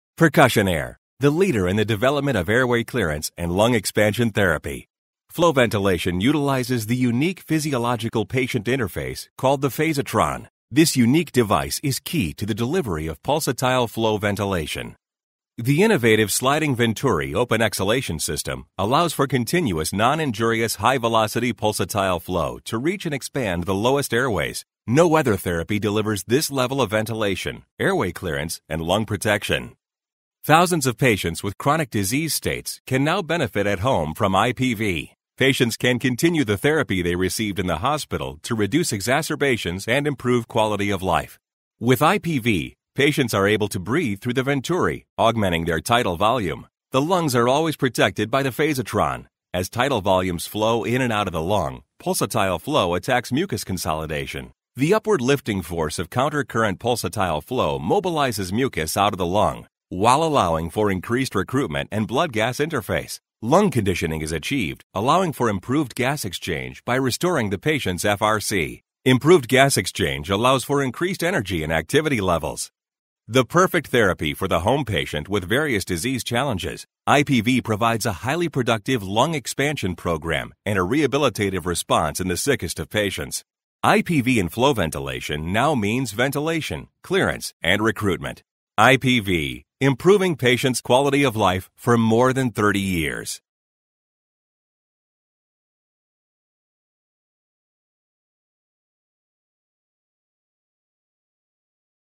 Percussionaire High Frequency Ventilation
Learn about our patented Phasitron technology, and our Intrapulmonary Percussive Ventilation devices providing therapy that works to clear airways of mucus and debris. Our products are used in all clinical environments from homecare to acute care and support patients from neonatal to large adults and are designed to help rehab teams successfully manage exacerbations and extended lengths of stays and are trusted in homecare, military, hospital, and transport applications.